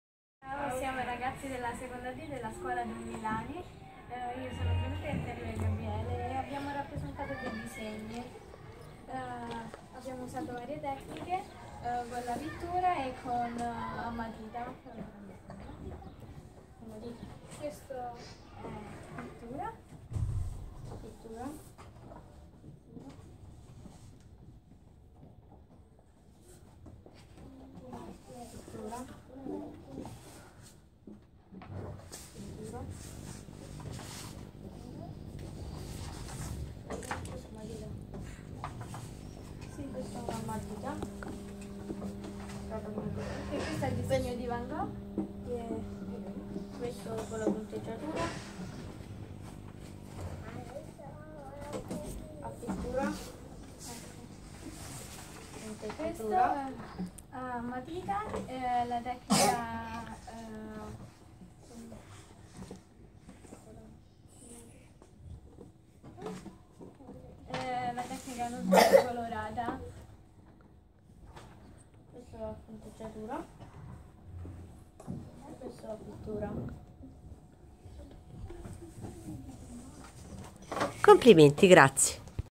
Gli impressionisti e le copie dei quadri
Ragazze/i descrivono a bambine/i della scuola dell'infanzia le copie dei quadri realizzate per il "ponte artistico" con la Slovacchia ed illustrano le varie tecniche utilizzate.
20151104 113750